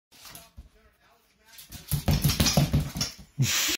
Pup Gone Wild!
Funny little video of a dog and his amusing toy antics.